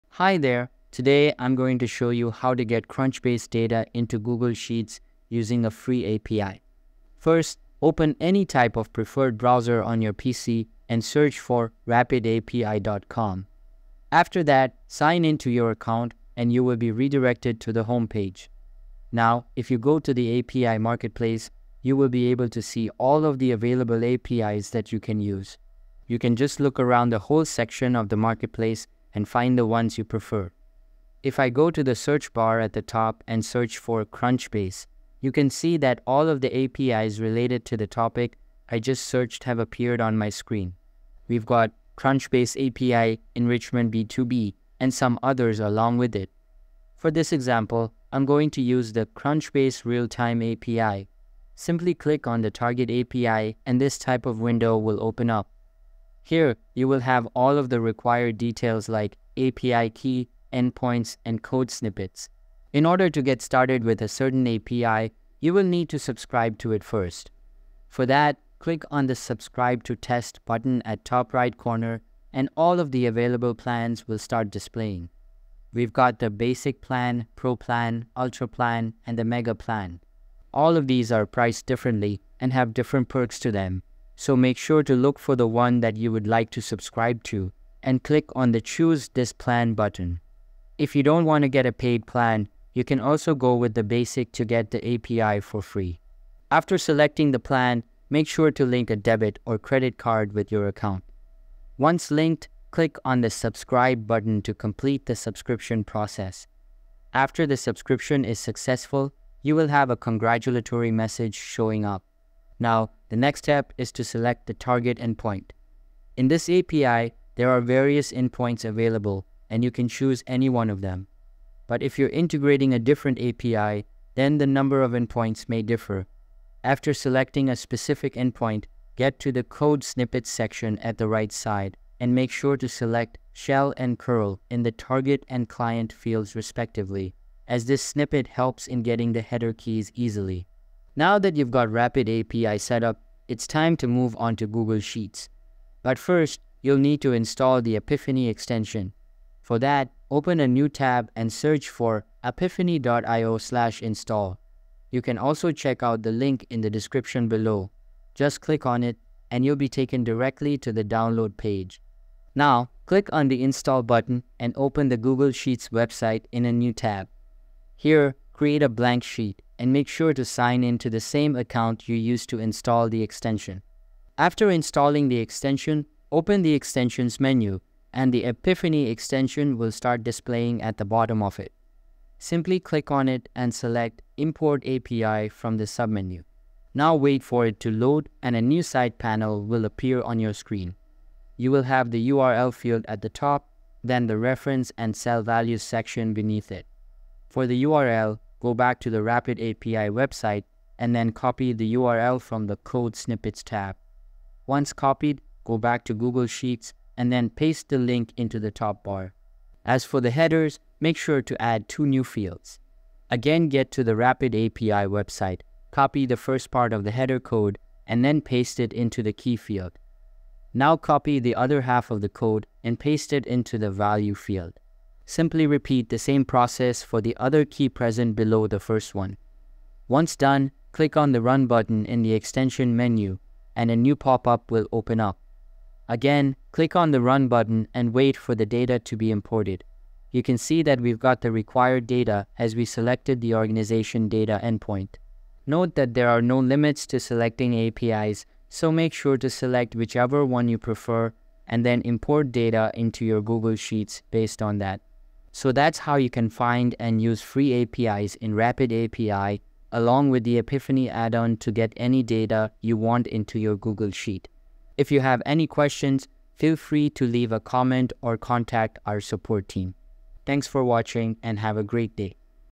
How To Import Crunchbase Data into Google Sheets Using a Free API
🎥 Simplify your business research and startup tracking by importing Crunchbase data directly into Google Sheets using a free API and Apipheny.

Apipheny is a free Google Sheets add-on that makes connecting APIs to your spreadsheet simple. By using a free Crunchbase API, you can import company details, funding rounds, industry categories, and investment data into Google Sheets, helping you track startups, research businesses, and monitor industry trends—all in one place.

🌟 Key Benefits:

Access to Crunchbase Data: Import key business details such as company names, funding rounds, investors, market categories, and more directly into Google Sheets.
Customizable Data Filters: Search by company names, industry sectors, funding stages, or regions to focus on the data that matters to your business research.
Automated Data Refresh: Set up automatic imports to keep your business research data updated, ensuring you stay ahead in competitive market analysis.

📚 What You’ll Master:

Finding the Right Crunchbase API: Learn how to find a free API that provides access to company profiles, startup funding rounds, and industry insights.
Getting Your API Key: Step-by-step guidance on how to sign up for a Crunchbase API and retrieve your API key for accessing company data.
Connecting Apipheny to Google Sheets: Detailed instructions on how to install Apipheny and link it to your chosen Crunchbase API for seamless data imports.
Importing and Managing Data: Discover how to import Crunchbase data into Google Sheets, filter by industry, funding stage, or location, and organize it for in-depth business analysis and investment tracking.